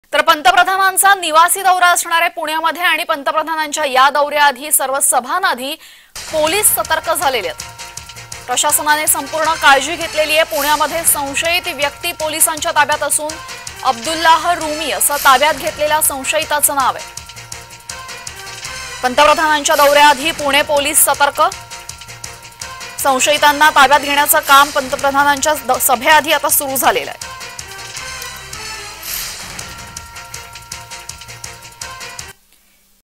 Narendra Modi | पंतप्रधान मोदींच्या सभेआधी पोलीस सतर्क | Marathi News
पंतप्रधान मोदींच्या सभेआधी पोलीस सतर्क
पुण्यात संशयित व्यक्ती पोलिसांच्या ताब्यात
अब्दुलाह रुमी असं ताब्यात घेतलेल्या संशयिताचे नाव